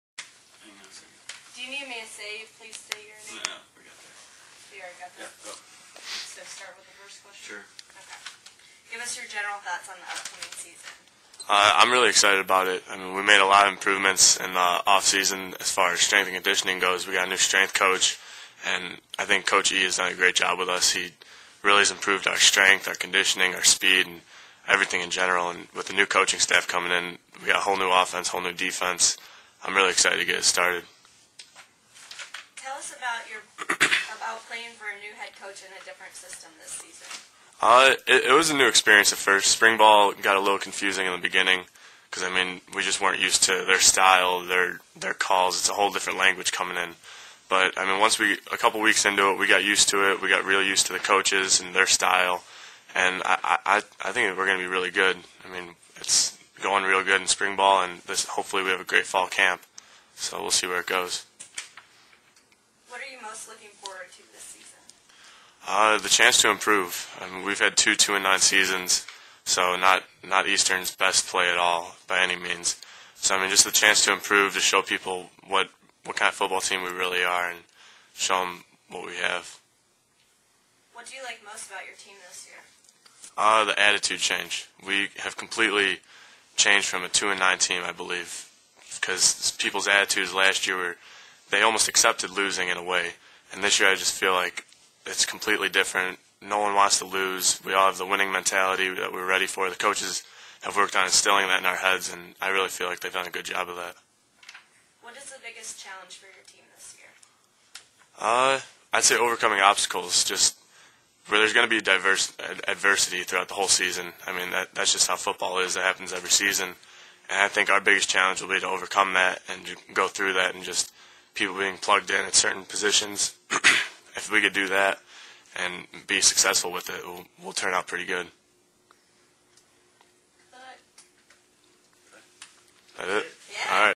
Eastern Illinois QB Jimmy Garoppolo - 2012 OVC Football Media Day
Eastern Illinois junior quarterback Jimmy Garoppolo discusses the upcoming season at the 2012 OVC Football Media Day.